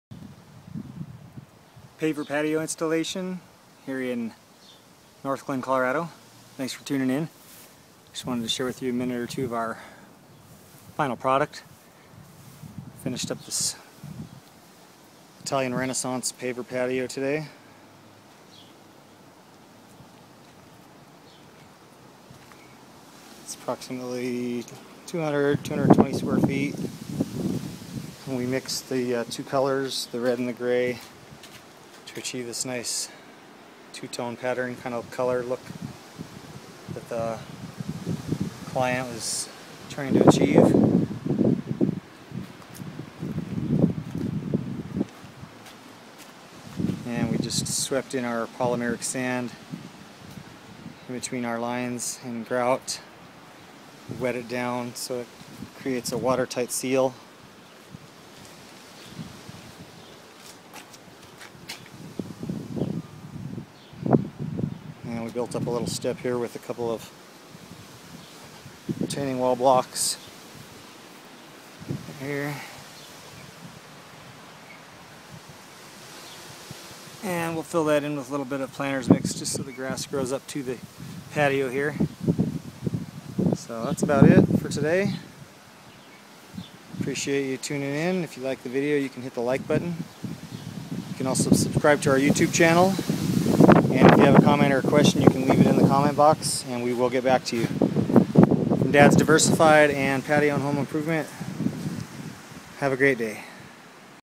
Paver Patio Installation Paver Patio Completed
We just finished this paver patio installation in Northglenn Colorado. First we graded the dirt for proper drainage and compacted a layer of road base over the native soils. We installed a commercial grade weed barrier over the base and used sand to drop the paver onto. We used a polymeric sand over the pavers to create a water tight seal. Call or leave a comment or question in the comment box for a free estimate in the metro Denver area.